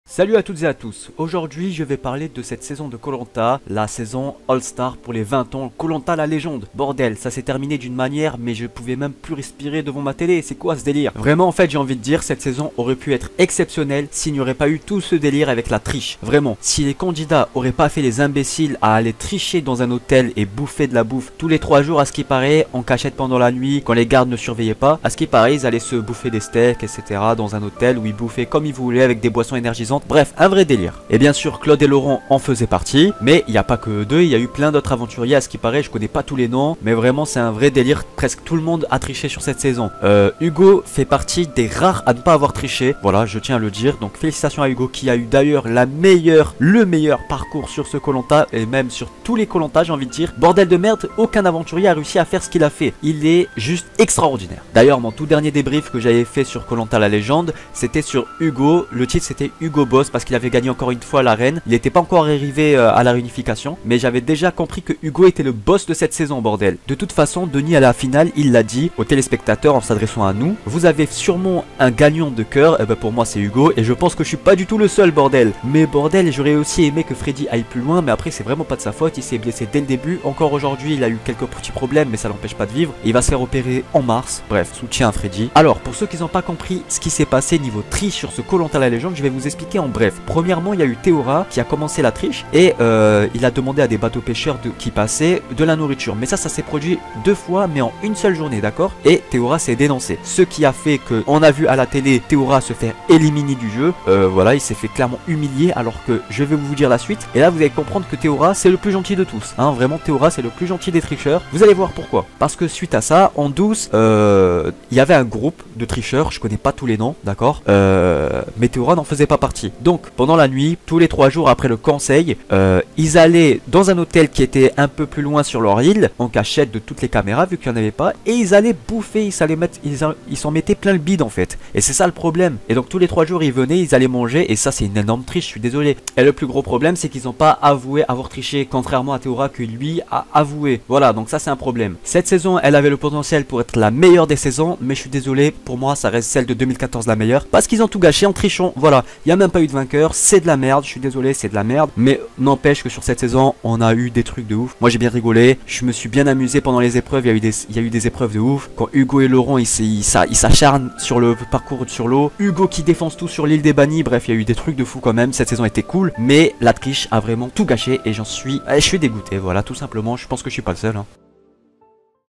Koh lanta la légende la finale - Cette saison humiliante
Koh lanta la légende, je suis déçu de plusieur aventurier qui ont fait n'importe quoi.
Respect à Hugo